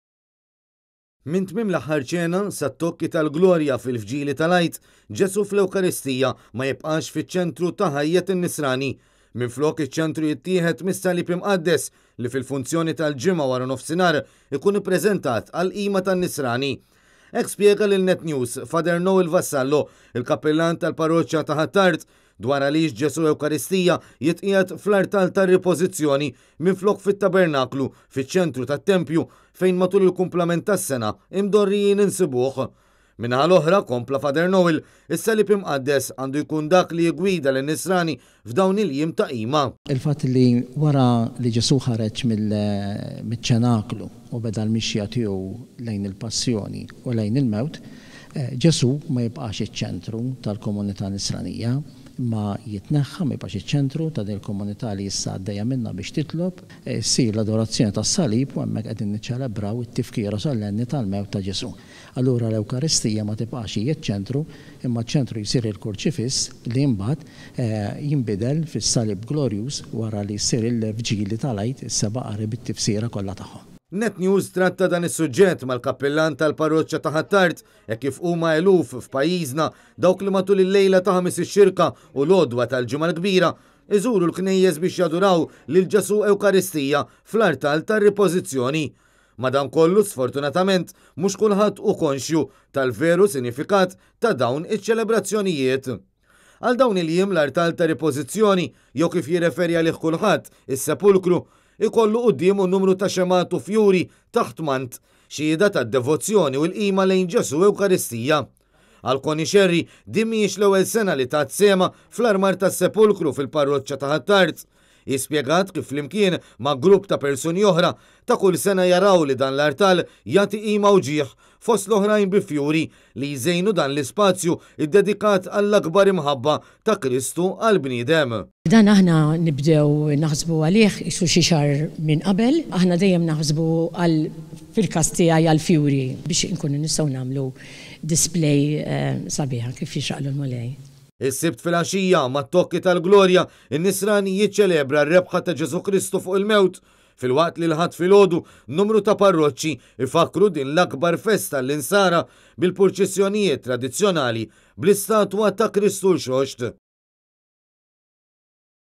IS-SALIB IMQADDES FIĊ-ĊENTRU TAL-ATTENZJONI GĦAN-NISRANI
Minn tmiem l-Aħħar Ċena sat-tokki tal-Glorja fil-Vġili tal-Għid, Ġesù fl-Ewkaristija ma jibqax fiċ-ċentru ta’ ħajjet in-Nisrani. Minflok, iċ-ċentru jittieħed mis-Salib Imqaddes, li fil-Funzjoni tal-Ġimgħa waranofsinhar, ikun ippreżentat għall-qima tan-Nisrani.